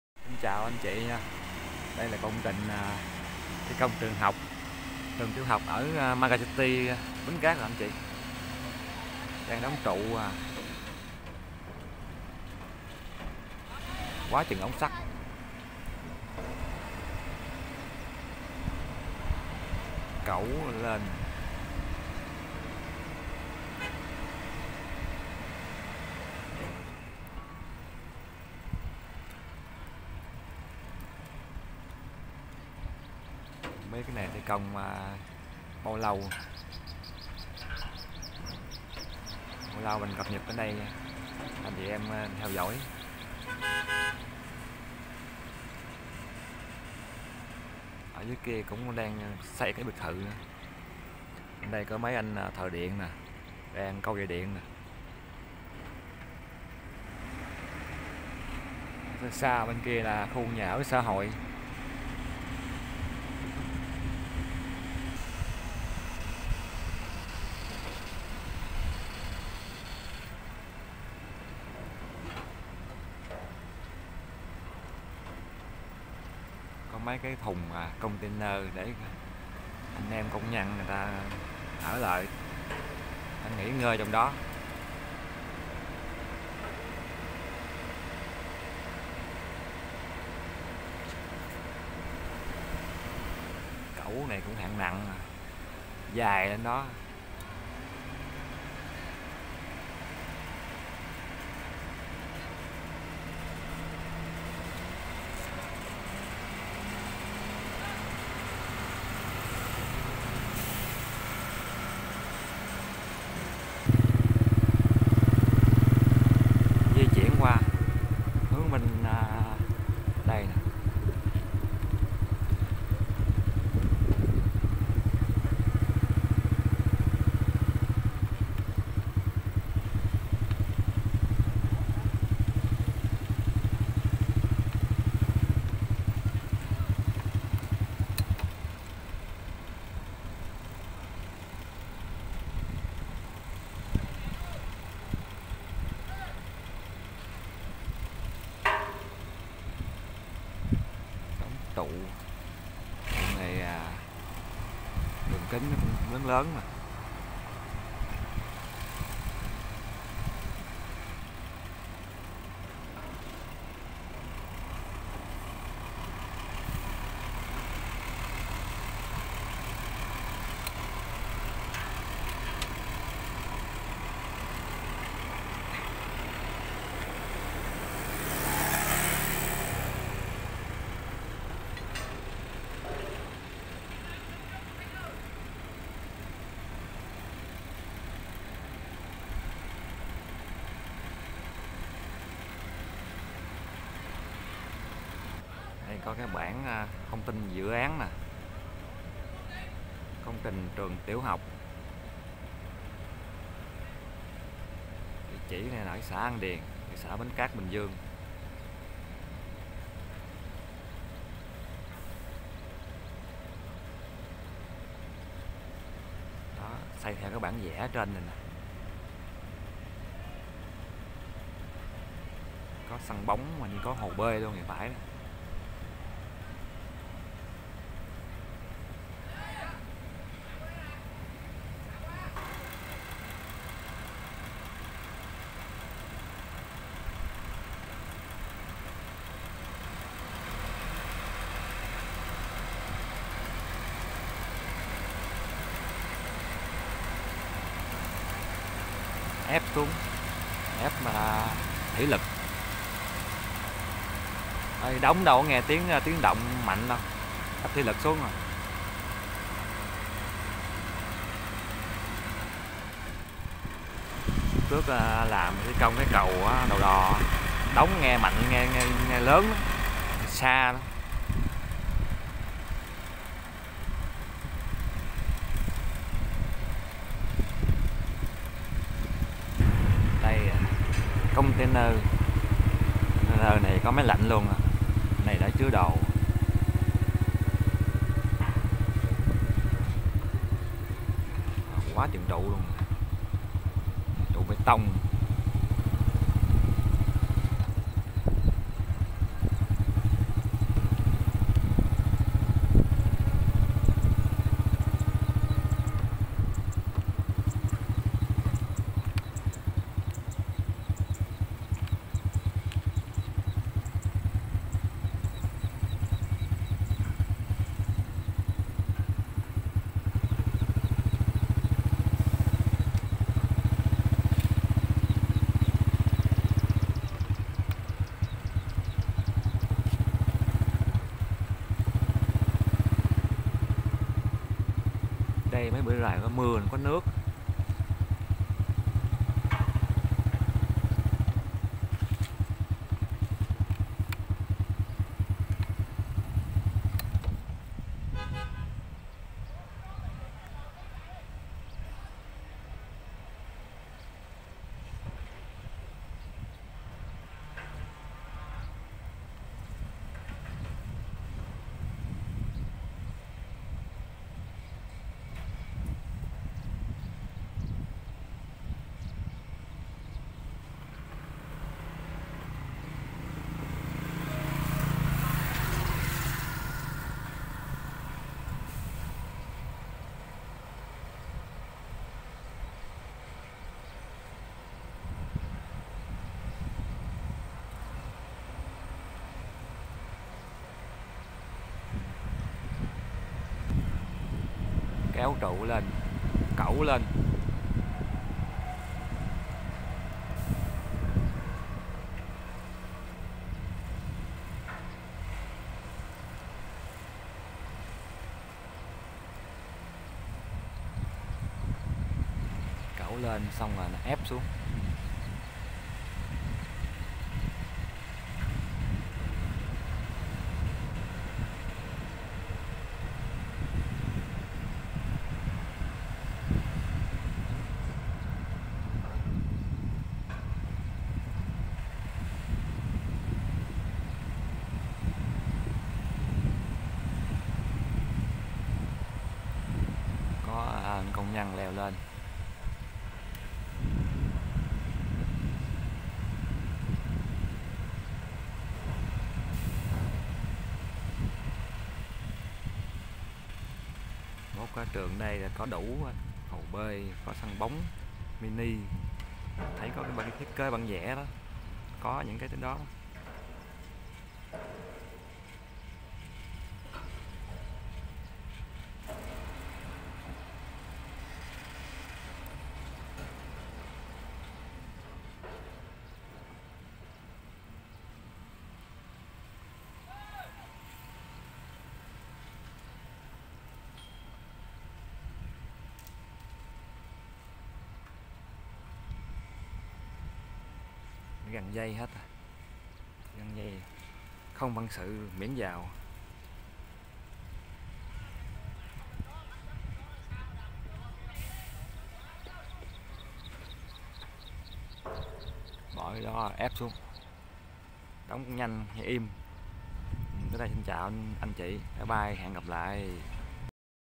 mới,dự án thi công trường học ở mega city, bến cát
cập nhật dự án thi công xây trường học ở mega city cầu đò 2 bến cát Bình Dương